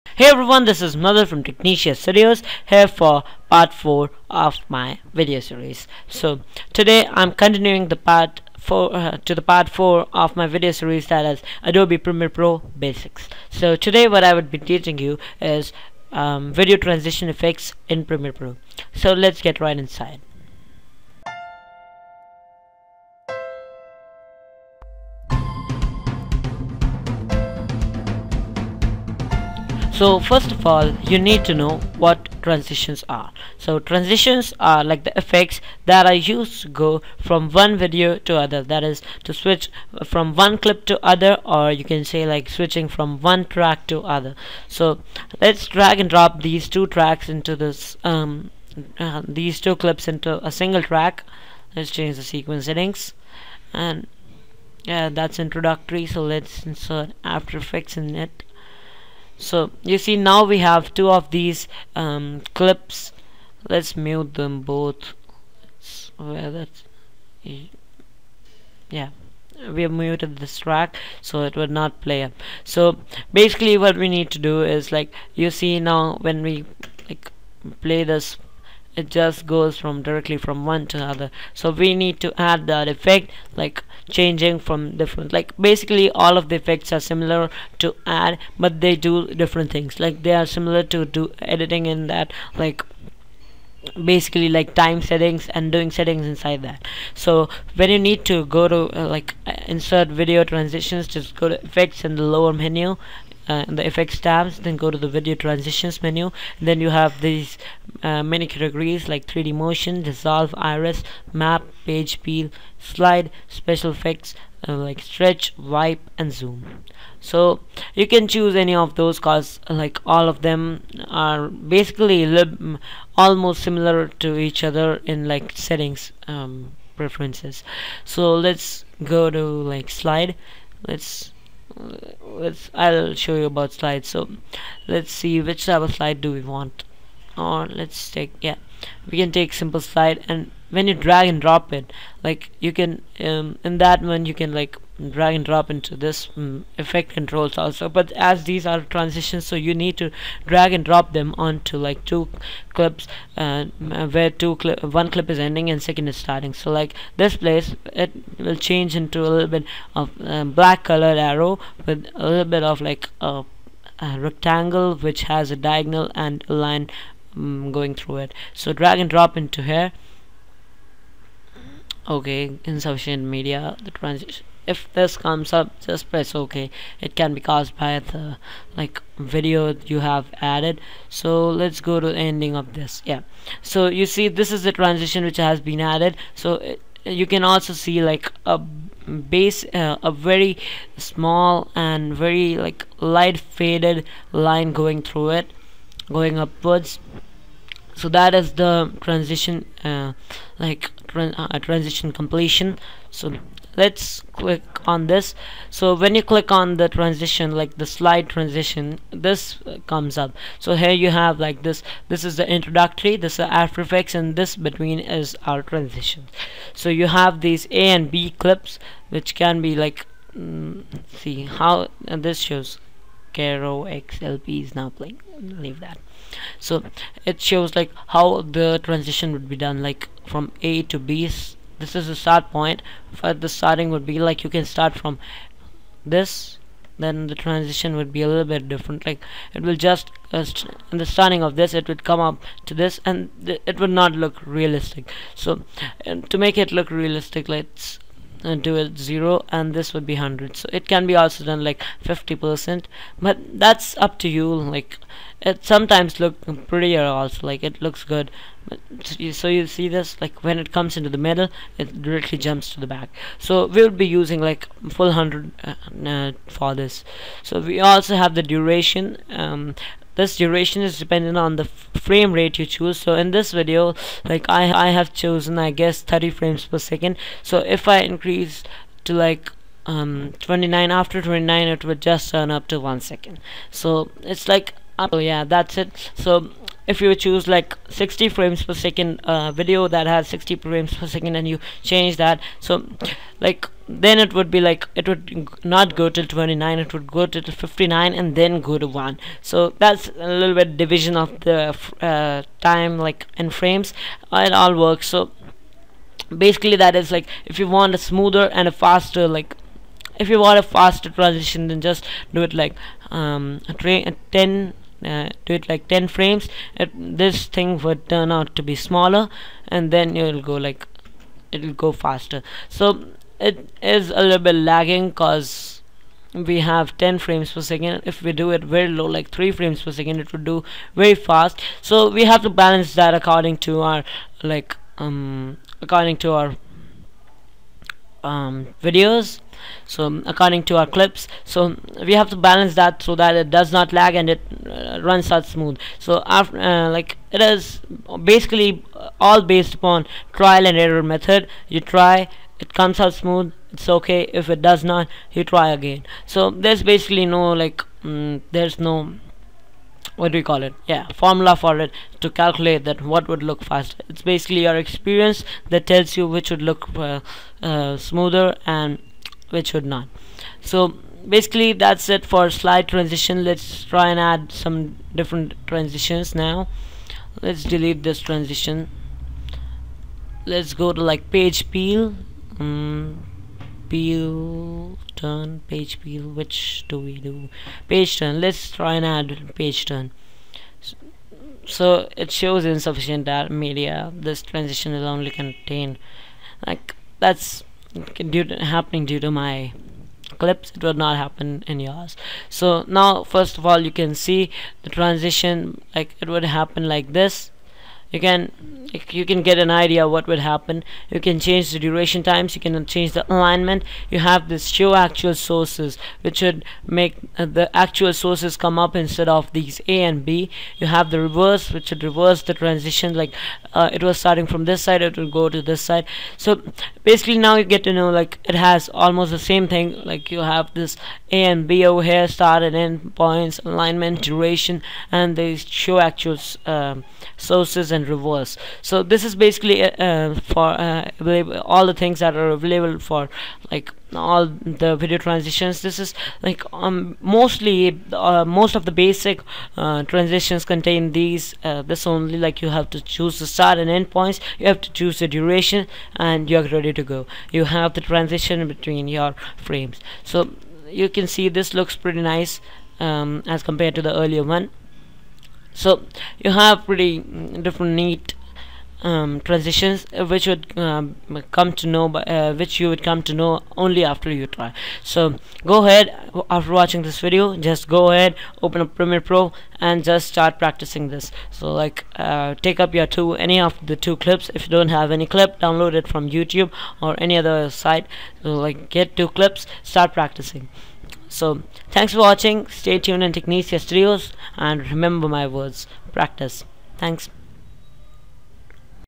Part 4: Video Transition Effects | Adobe Premiere Pro Basics
Switching between two different clips is a very basic and important component of learning Video Editing, So in this video we will be considering the Video Transitioning Effects of Premiere Pro